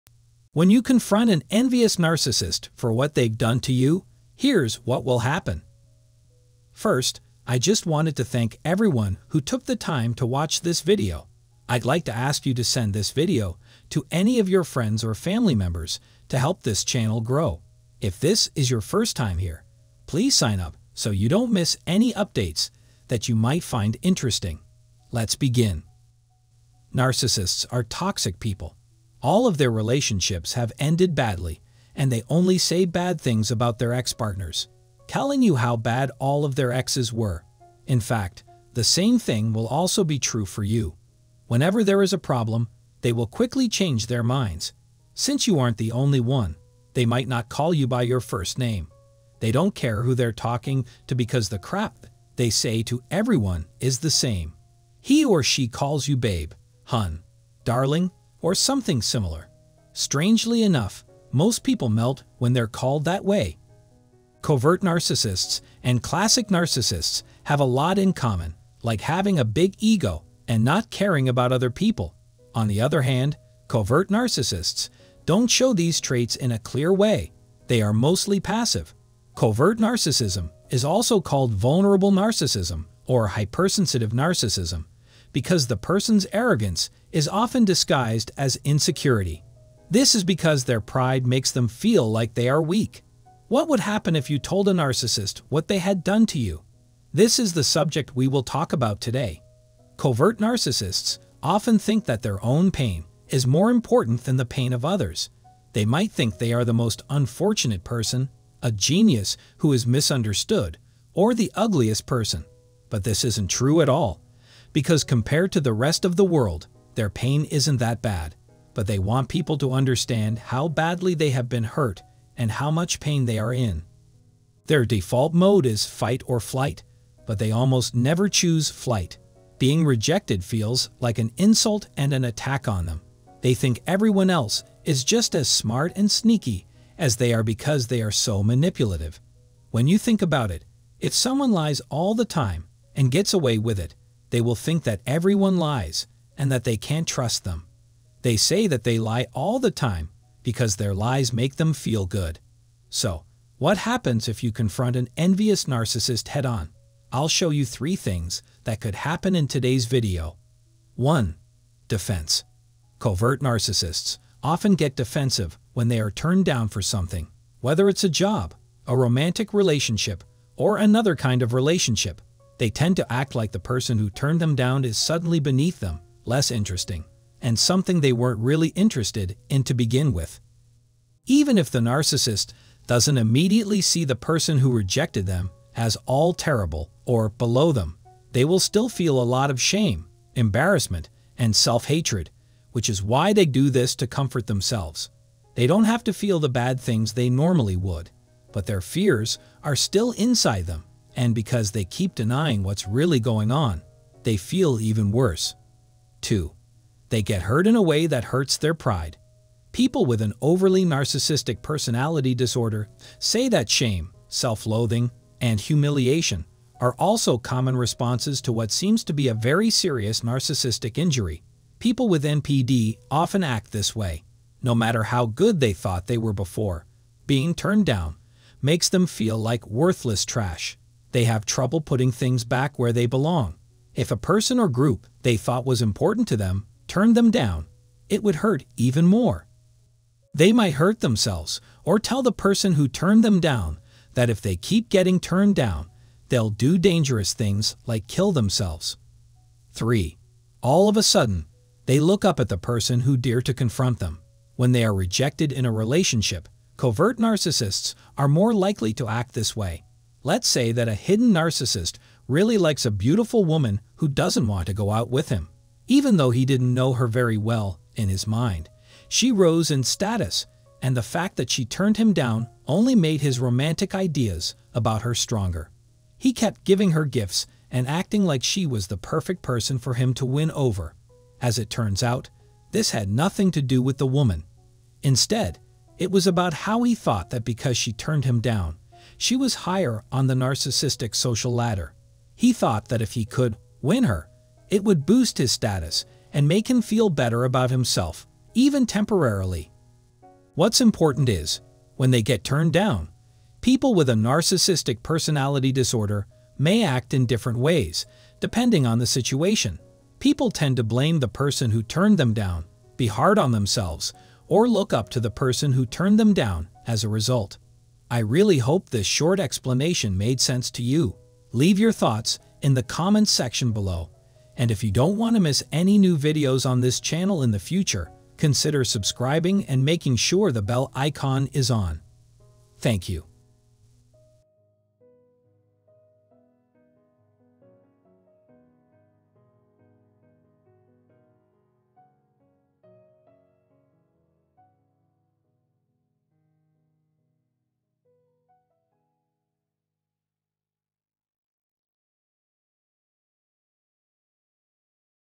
When You Confront An ENVIOUS Narcissist For What They've Done To You, Here's What Will Happen | NPD
When A Narcissist Feels They Can't Get What They Want From You, They'll DO THIS! | NPD | Narcissist

This is what a narcissist does when you leave them and refuse to give them what they want.

First, I'd like to say thanks to everyone who watched this video. Let's begin. NPD

Please help this channel to gain more vibrations by sharing this video with your family and friends.

0:00 - Opening
2:41 - Defense
3:26 - They get hurt in a way that hurts their pride
4:09 - All of a sudden, they look up at the person who dear to confront them
5:18 - Ending